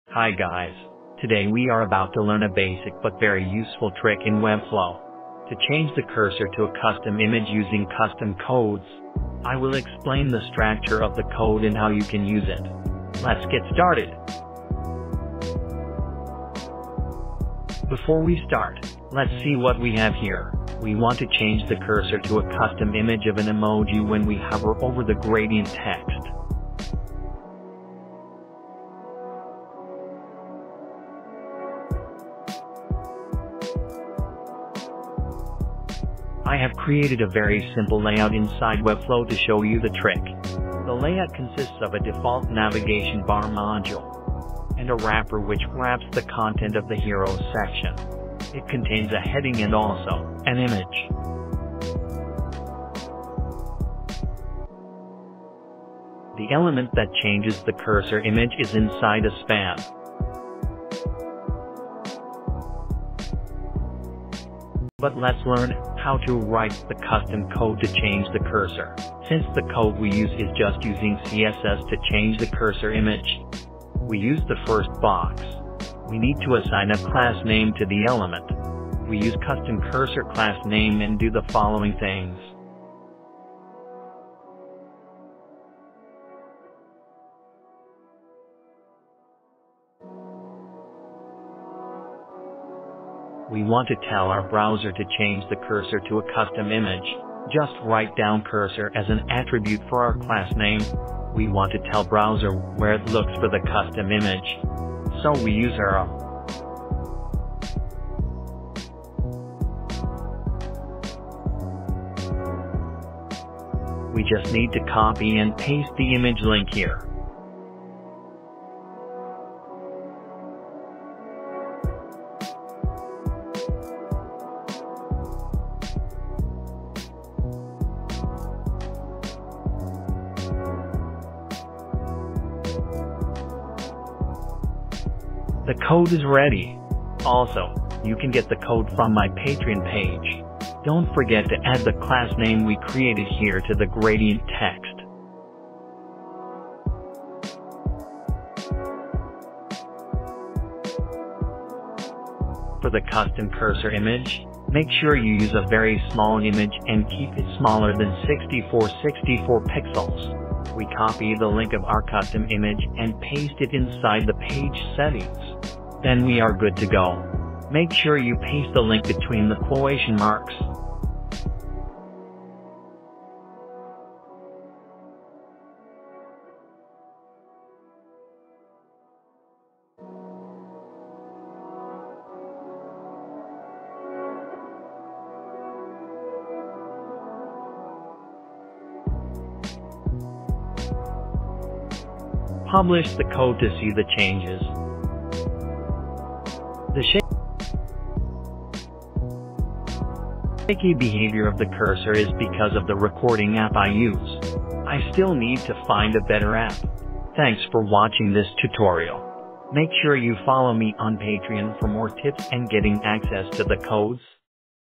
Webflow Tutorial: Use a Custom Cursor
In this tutorial, we learn how to use a custom image as cursor.